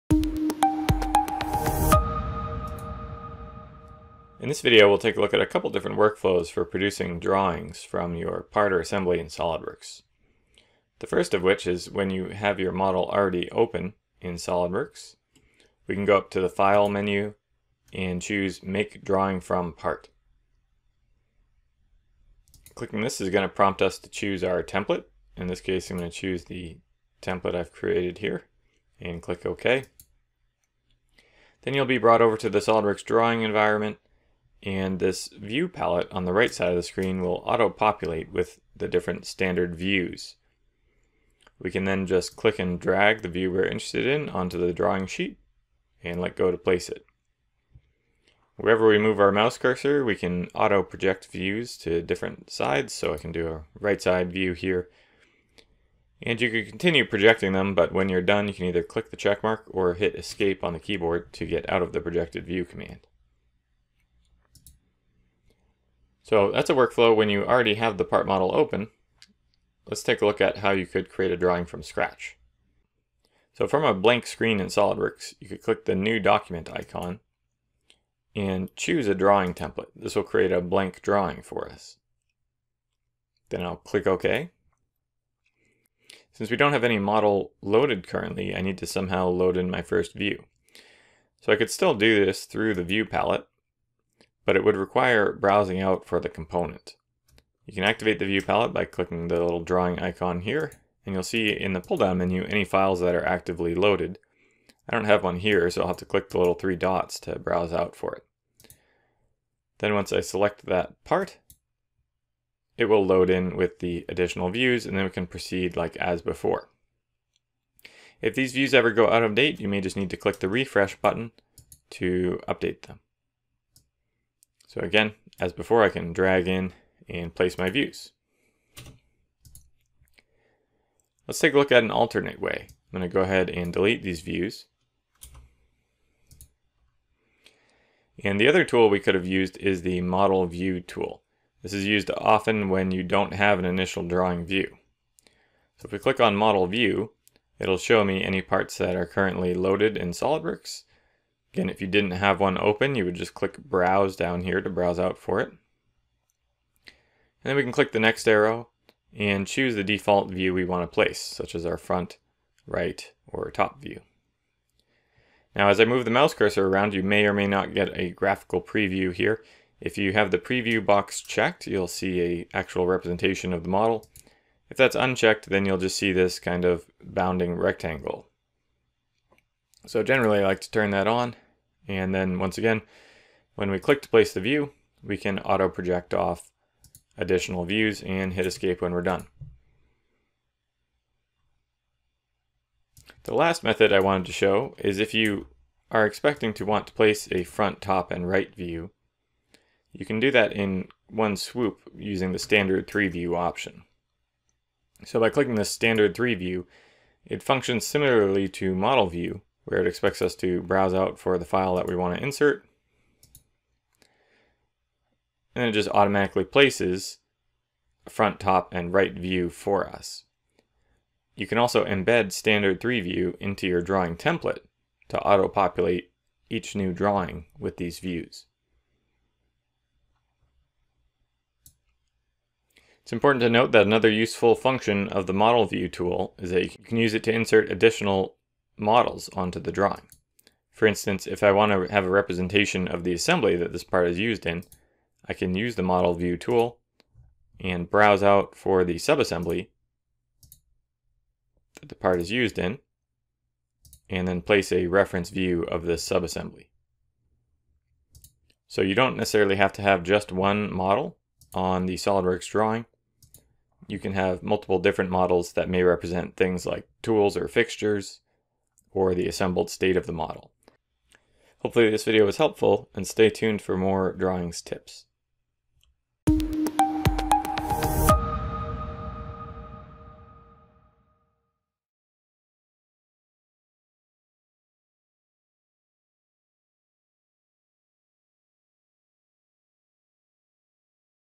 Placing Drawing Views in SOLIDWORKS
Learn multiple methods for placing drawing views in SOLIDWORKS. From the part or assembly, the "Create Drawing from Part/Assembly" command automatically initiated drawing creation and populates the View Palette with available views.

Alternate approaches include: creating a new drawing and placing views with the Model View command, View Palette, or Standard 3 View functionality.

Presented by Ryan Navarro